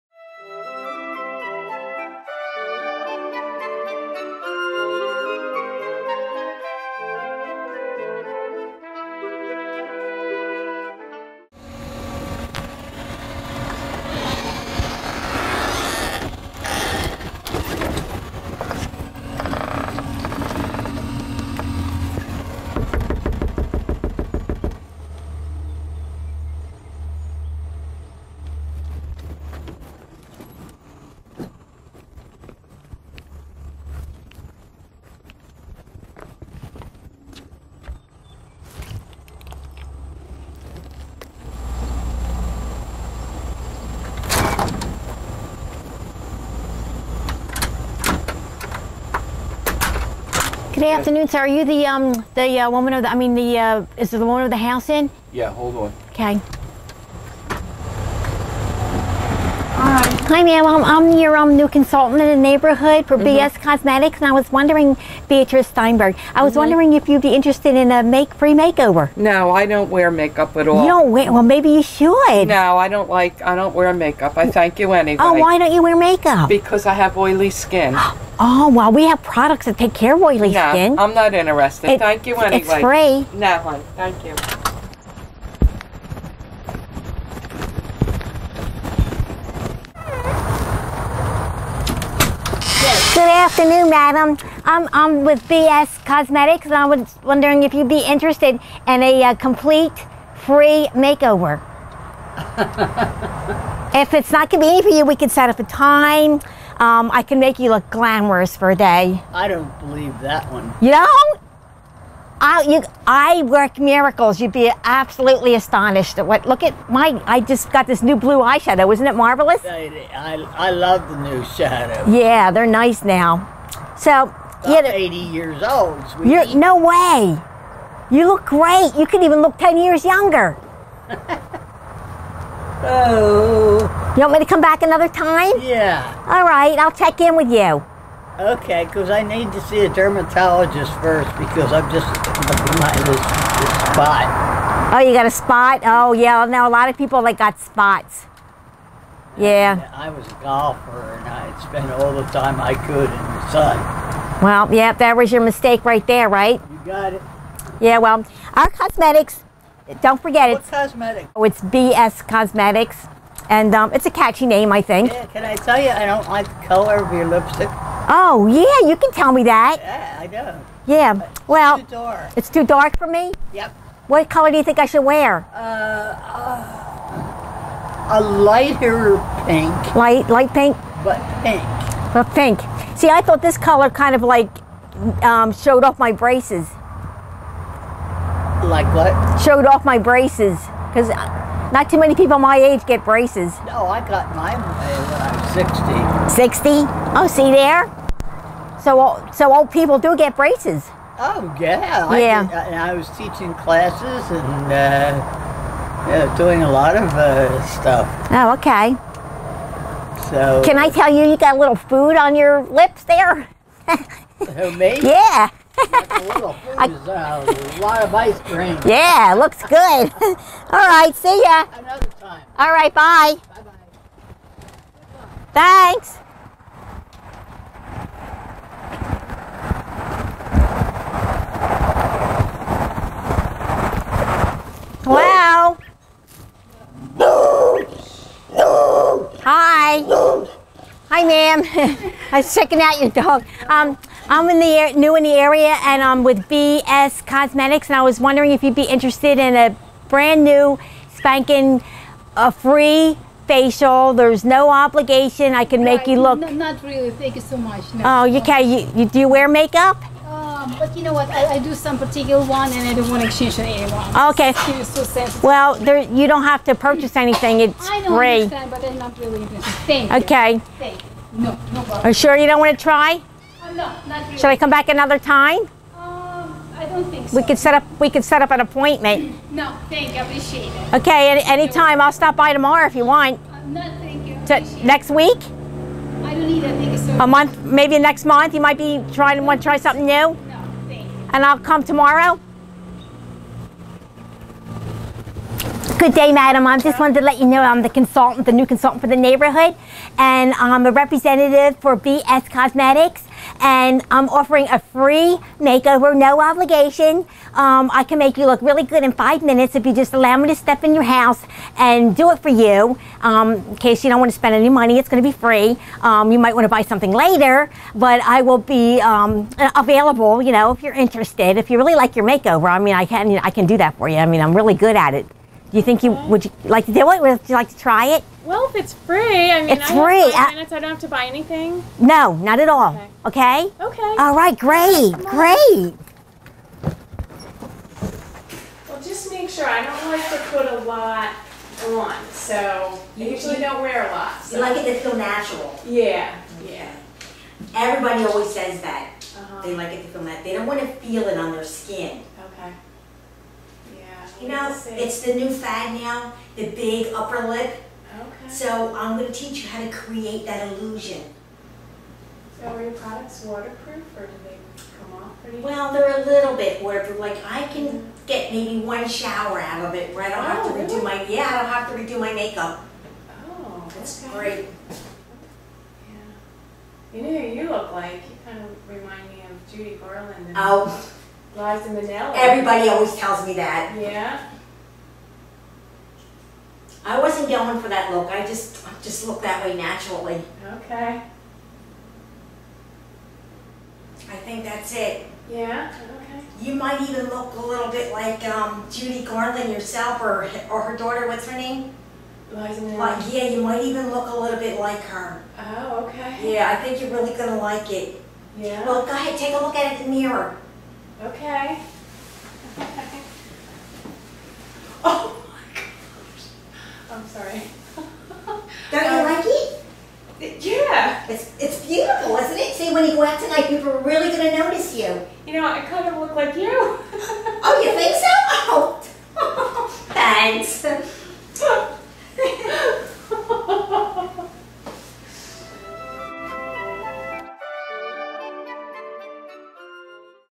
Bull S**t Cosmetics
Beatrice's new job, selling cosmetics door to door.